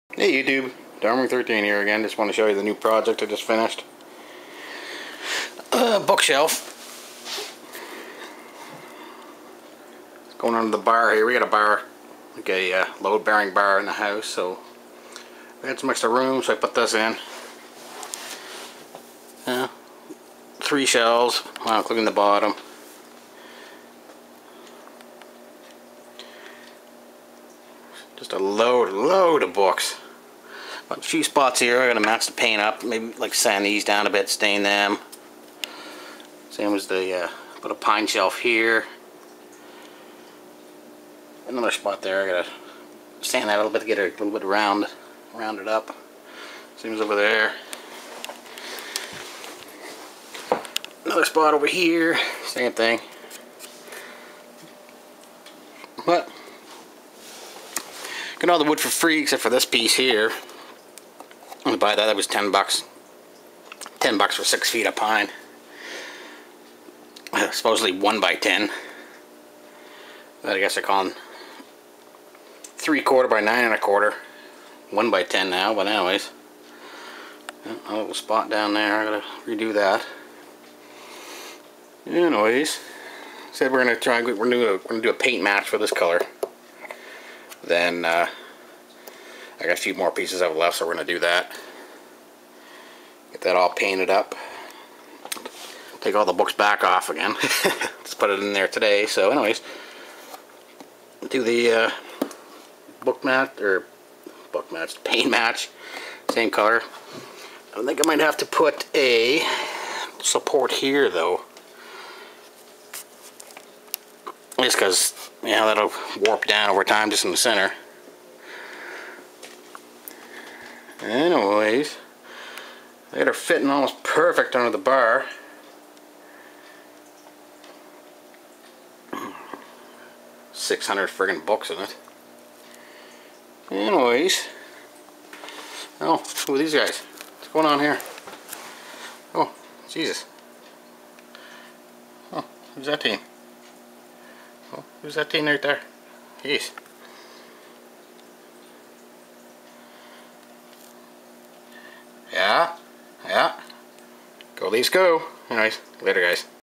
Book shelf project we just finished.
A project myself and Mrs. Rig just finished.   Cost us all of about 10 bucks to do, just for the center shelf.  Made out of old waterbed frame's..hell, it was free!!
We have tons of books, yes we read a lot!  A few mistakes were made, but ...carpentry isn't easy, and I am a carpenter...with still a lot to learn!  Plus an amazing few figures show up an the end!  Just wait until the next project!!